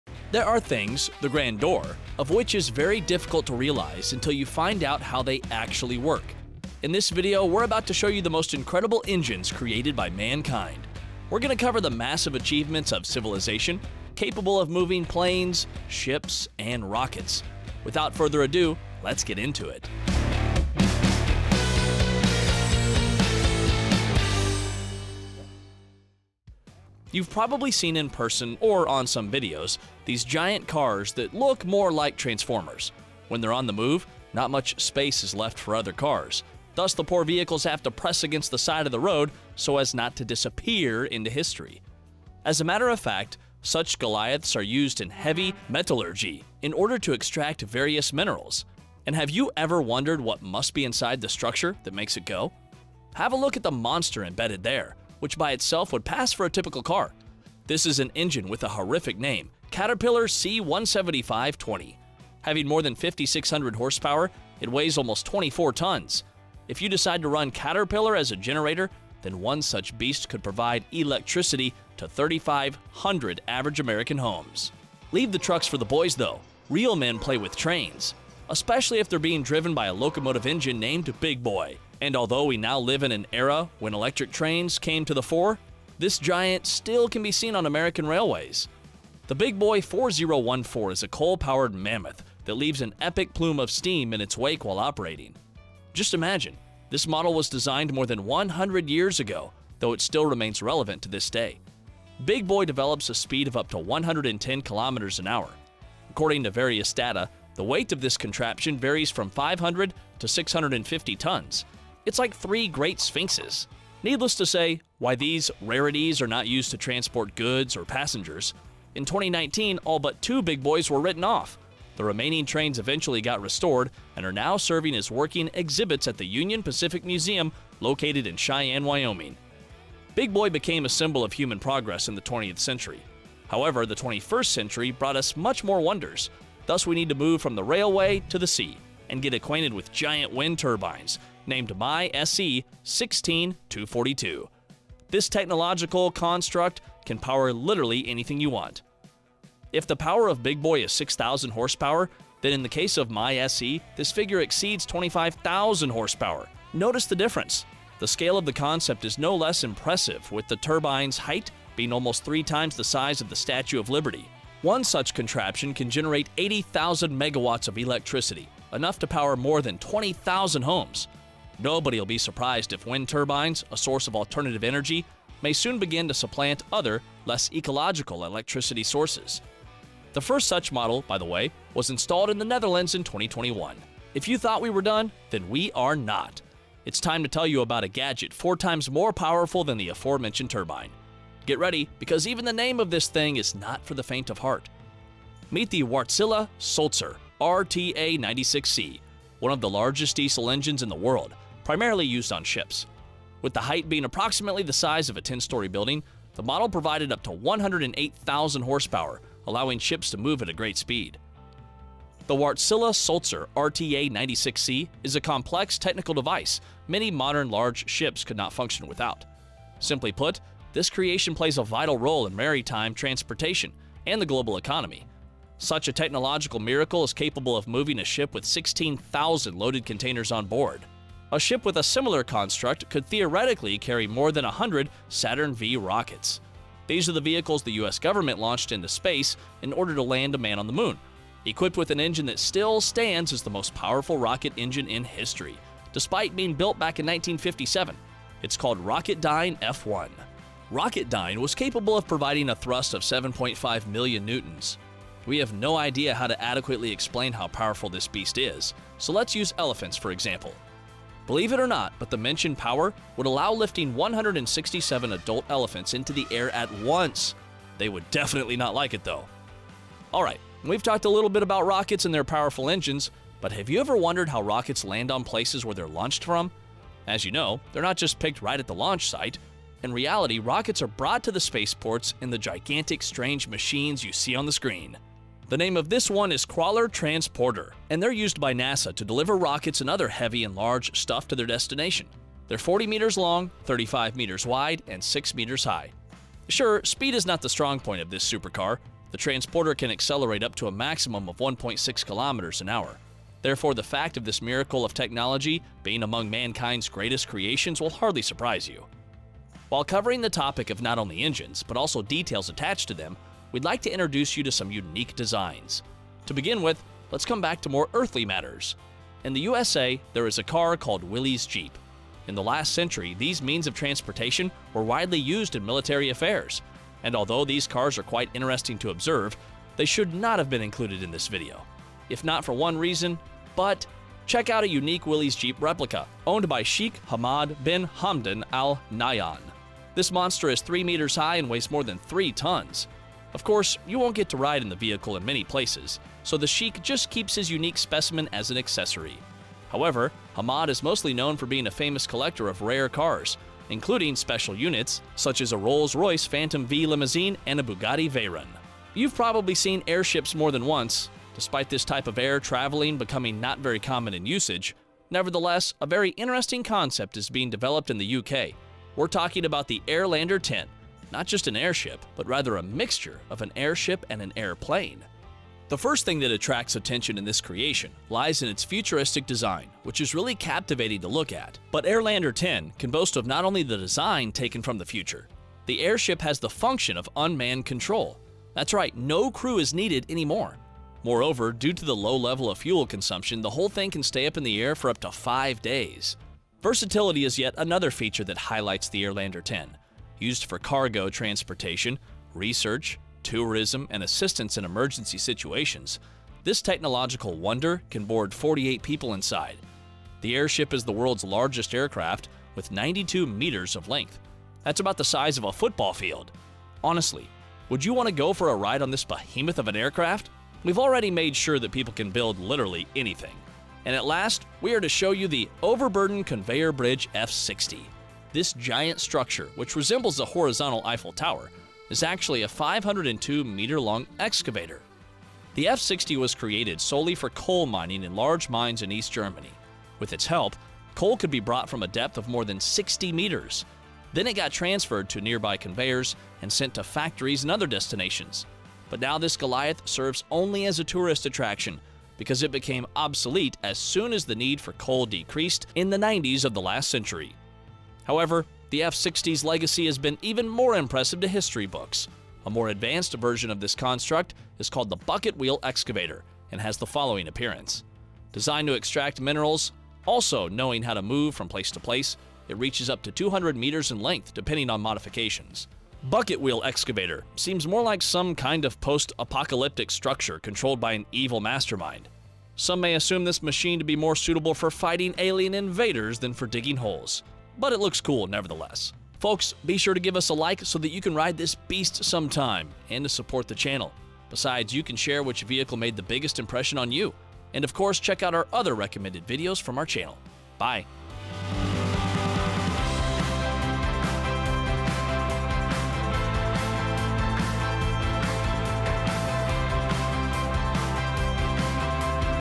WORLD'S BIGGEST ENGINES
There are things, the grandeur of which is very difficult to realize until you find out how they actually work. In this video, we are about to show you the most incredible engines created by mankind. We are going to cover the massive achievements of civilization, capable of moving planes, ships and rockets. Without further ado, let's get into it!